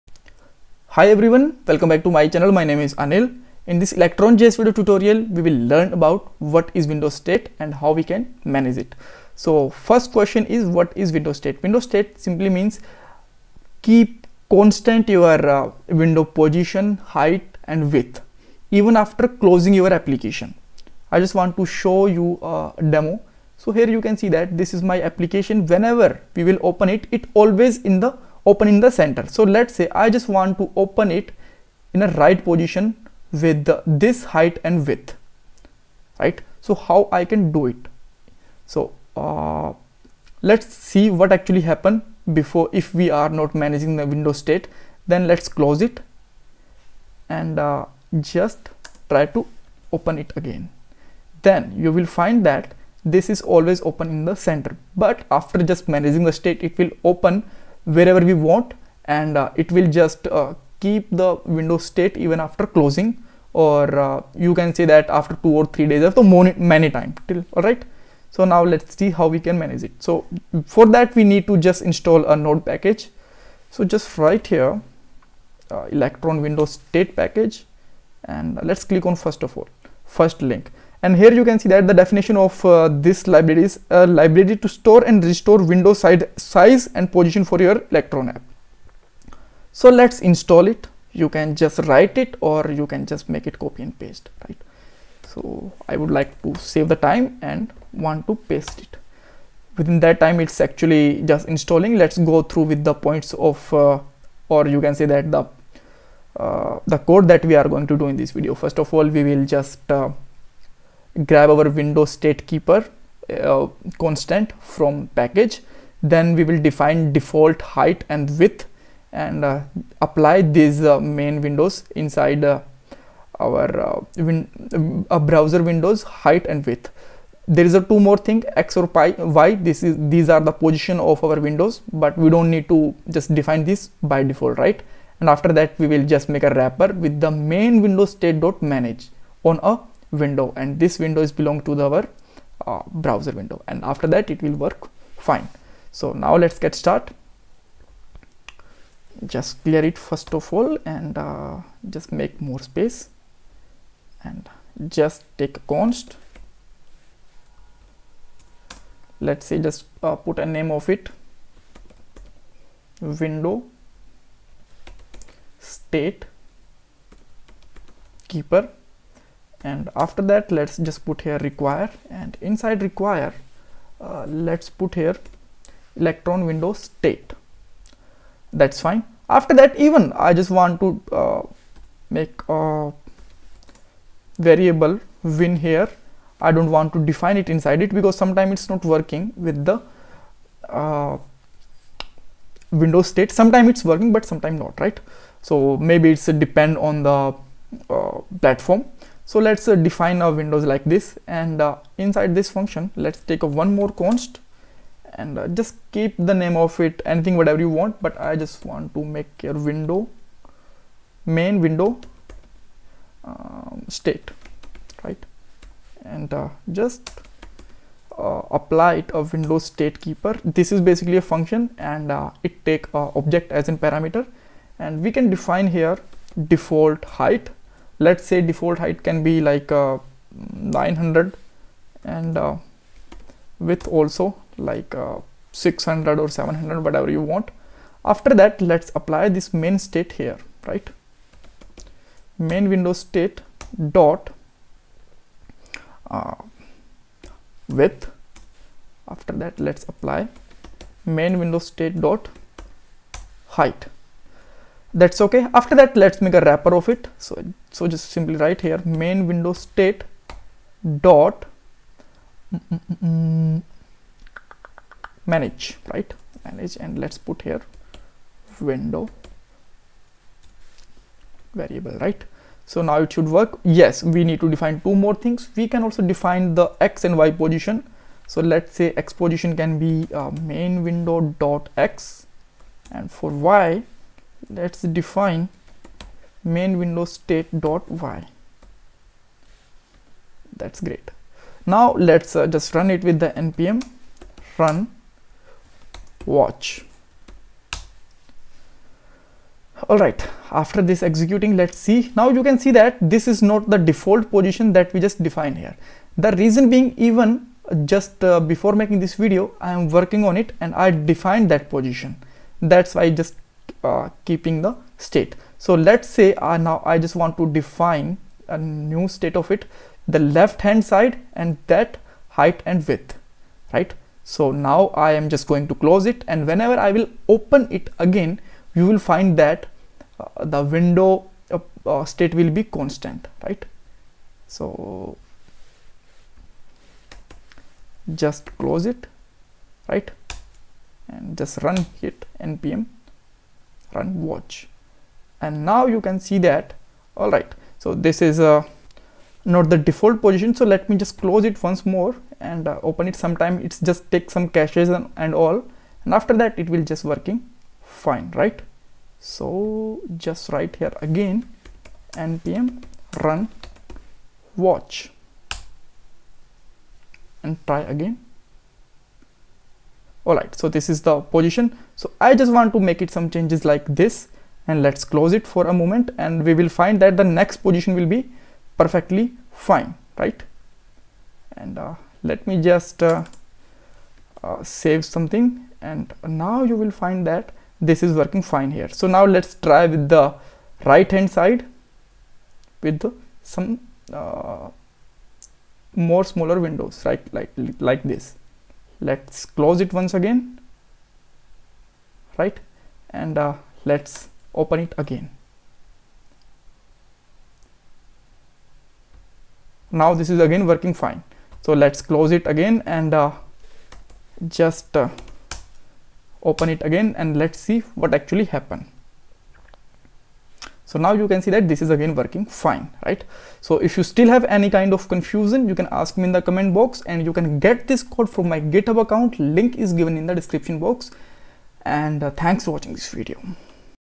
Electron js tutorial for beginners # Window State
Window State
What is window state?
Add node package for window state
Apply State and test